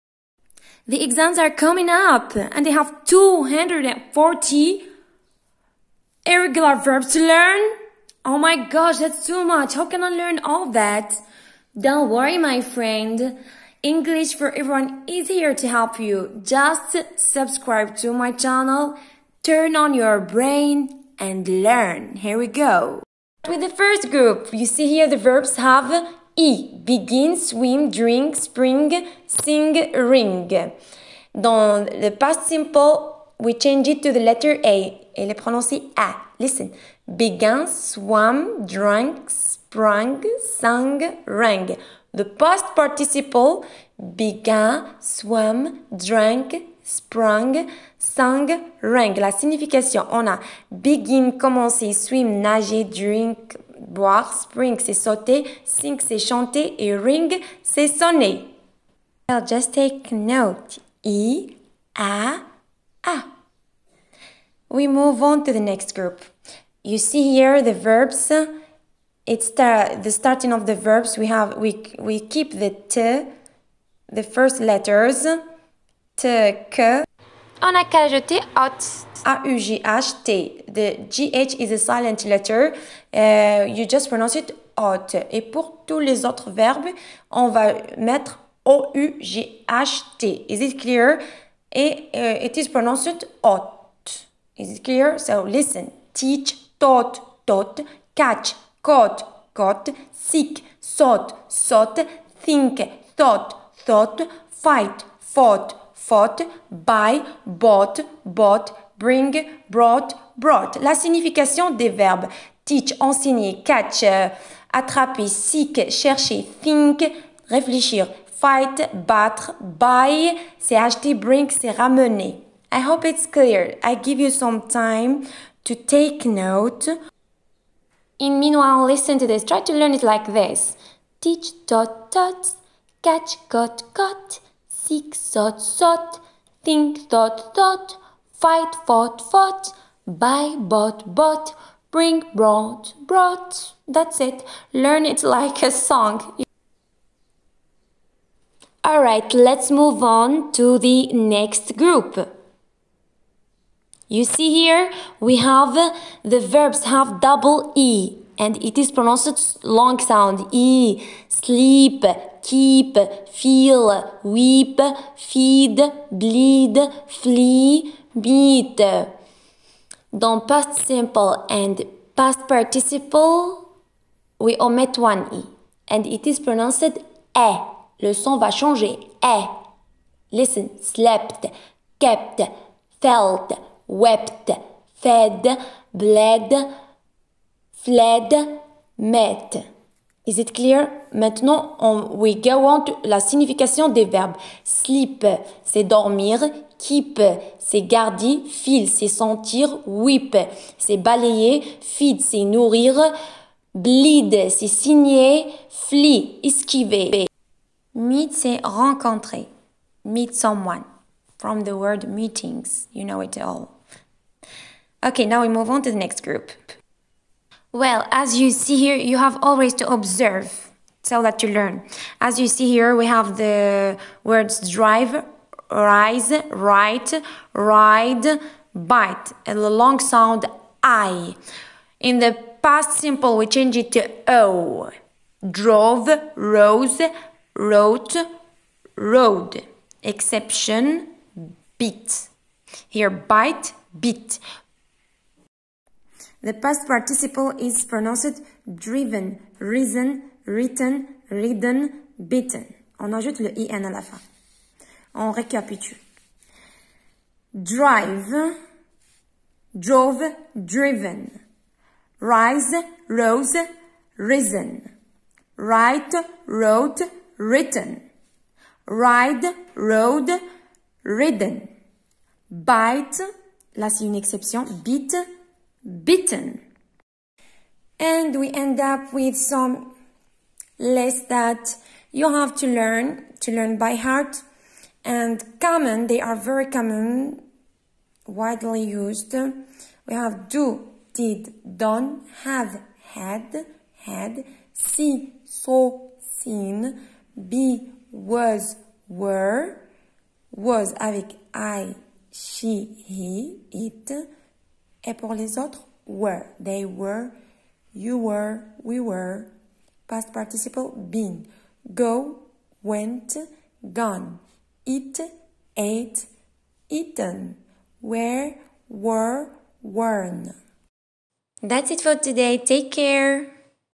Irregular verbs in 6minutes👉les verbes irréguliers- BEM BAC
Apprenez les verbes irréguliers dans 6minute with Rosa
Je recommande cet vidéo pour les élèves de 4Am 3AS=  BEM et  BAC